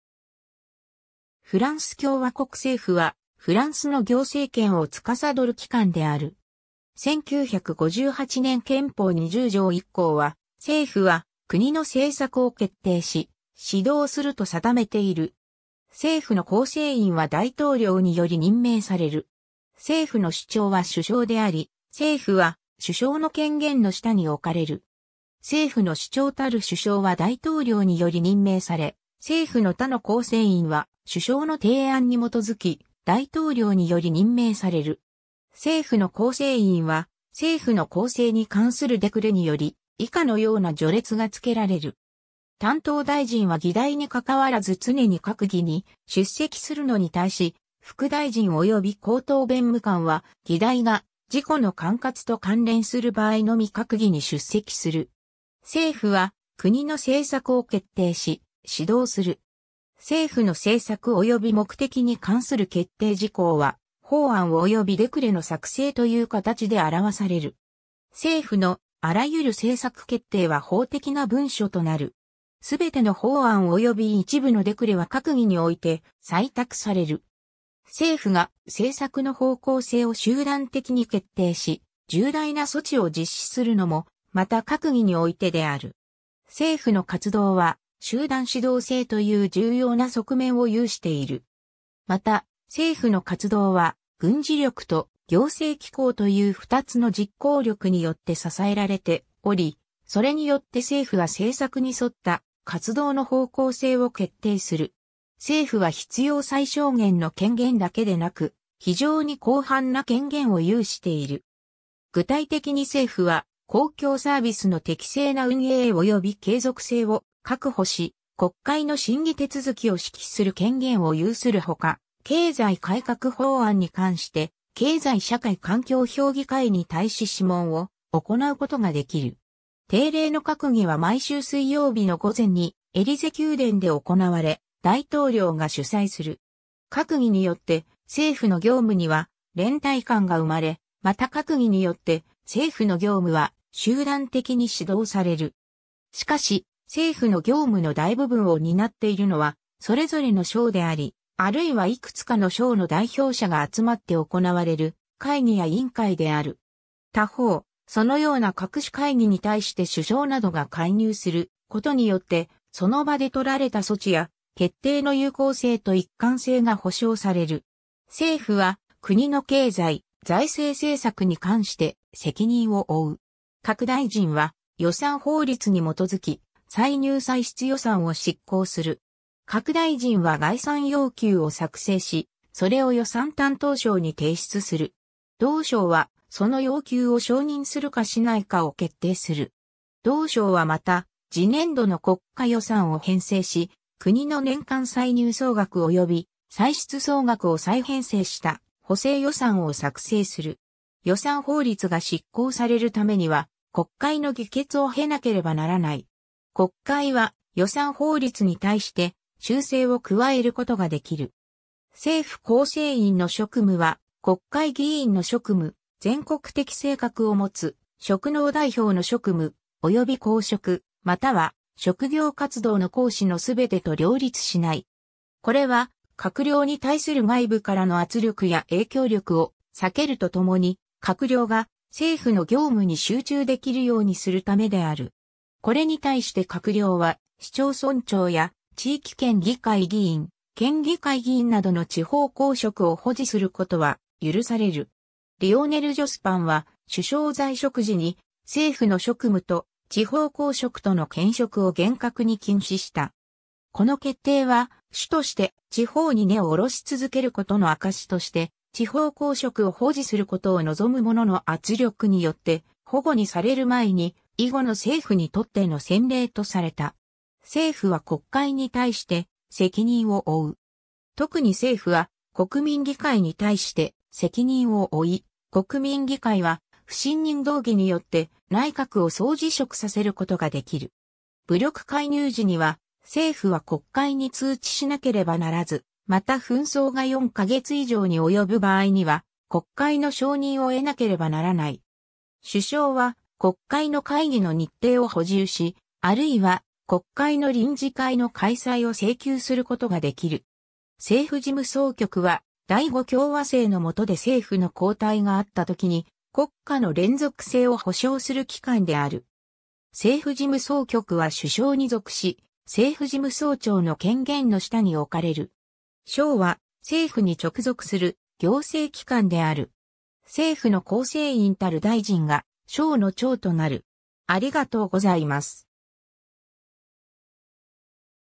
政府 (フランス第五共和政)
フランス共和国政府（フランスきょうわこくせいふ、フランス語: Gouvernement de la République française）は、フランスの行政権（執行権）を司る機関である。
1958年憲法（第五共和政憲法、以下「憲法」と略す）20条1項は、政府は国の政策を決定し、指導すると定めている。
 政府の構成員は大統領により任命される。
政府の首長は首相であり、政府は首相の権限の下に置かれる。
 政府の首長たる首相は大統領により任命され（憲法8条1項）、政府の他の構成員は首相の提案に基づき、大統領により任命される（同条2項）。
政府の構成員は政府の構成に関するデクレ（大統領令）により、以下のような序列が付けられる。
 担当大臣は議題にかかわらず常に閣議に出席するのに対し、副大臣および高等弁務官は議題が自己の管轄と関連する場合のみ閣議に出席する。
 政府は国の政策を決定し、指導する（憲法20条1項）。
政府の政策および目的に関する決定事項は、法案およびデクレ（政令）の作成という形で表される。
政府のあらゆる政策決定は法的な文書となる。
すべての法案および一部のデクレは閣議において採択される。
政府が政策の方向性を集団的に決定し、重大な措置を実施するのもまた閣議においてである。
政府の活動は、集団指導制という重要な側面を有している。
 また、政府の活動は軍事力と行政機構という2つの実行力によって支えられており、それによって政府は政策に沿った活動の方向性を決定する。
 政府は必要最小限の権限だけでなく、非常に広範な権限を有している。
具体的に政府は、公共サービスの適正な運営および継続性を確保し、国会の審議手続を指揮する権限を有するほか、経済改革法案に関して、経済社会環境評議会（Conseil économique, social et environnemental）に対し諮問を行うことができる。
 定例の閣議（Conseil des ministres）は毎週水曜日の午前にエリゼ宮殿で行われ、大統領が主宰する。
閣議によって政府の業務には連帯感が生まれ、また閣議によって政府の業務は集団的に指導される。
しかし、政府の業務の大部分を担っているのはそれぞれの省であり、あるいはいくつかの省の代表者が集まって行われる会議や委員会である。
他方、そのような各種会議に対して首相などが介入することによって、その場で取られた措置や決定の有効性と一貫性が保証される。
 政府は国の経済・財政政策に関して責任を負う。
各大臣は予算法律（loi de finances）に基づき、歳入歳出予算を執行する。
各大臣は概算要求を作成し、それを予算担当省（ministère chargé du budget）に提出する。
同省はその要求を承認するかしないかを決定する。
同省はまた、次年度の国家予算を編成し、国の年間歳入総額および歳出総額を再編成した補正予算を作成する。
 予算法律が執行されるためには、国会の議決を経なければならない。
国会は予算法律に対して修正を加えることができる。
 政府構成員の職務は、国会議員の職務、全国的性格を持つ職能代表の職務、および公職、または職業活動の行使のすべてと両立しない（憲法23条1項）。
これは、閣僚に対する外部からの圧力や影響力を避けるとともに、閣僚が政府の業務に集中できるようにするためである。
 これに対して閣僚は、市町村長や地域圏議会議員、県議会議員などの地方公職を保持することは許される。
リオネル・ジョスパンは首相在職時に、政府の職務と地方公職との兼職を厳格に禁止した。
この決定は、主として、地方に根を下ろし続けることの証しとして地方公職を保持することを望む者の圧力によって反故にされる前に、以後の政府にとっての先例とされた。
 政府は国会に対して責任を負う（憲法20条3項）。
特に政府は国民議会（下院）に対して責任を負い、国民議会は不信任動議（motion de censure）によって内閣を総辞職させることができる（憲法49条2項、50条）。
 武力介入時には政府は国会に通知しなければならず（憲法35条2項）、また紛争が4か月以上に及ぶ場合には国会の承認を得なければならない（同条3項）。
 首相は国会の会議の日程を補充し（憲法28条3項）、あるいは国会の臨時会（session extraordinaire）の開催を請求することができる（憲法29条1項）。
 政...